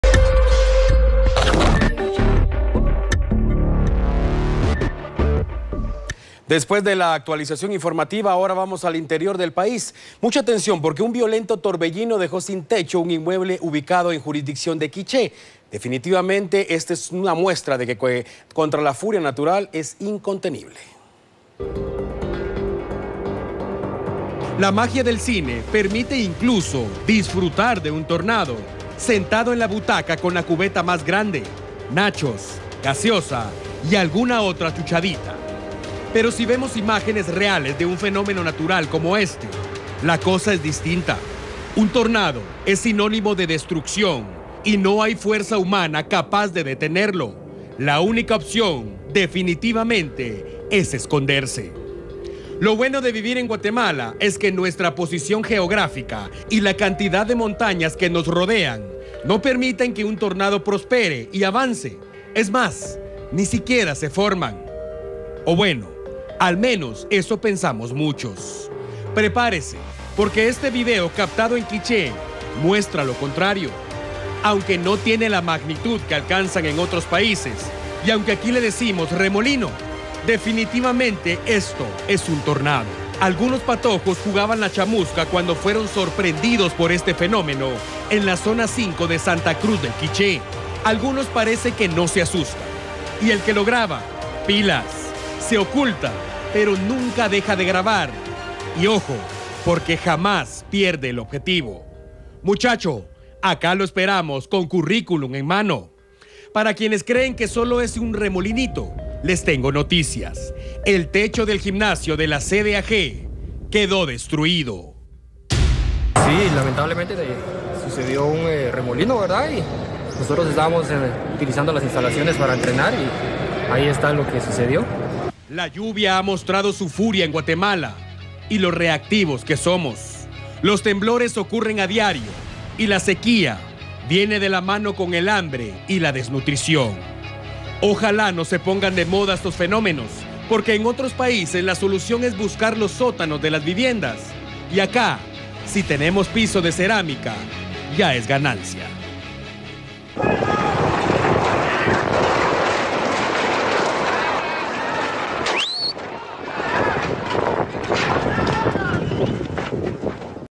Violento torbellino destruye techo de gimnasio en Quiché
Un violento torbellino dejó sin techo a un inmueble ubicado en jurisdicción de Quiché. Definitivamente esta es una muestra de cuando la furia natural ocurre es incontenible.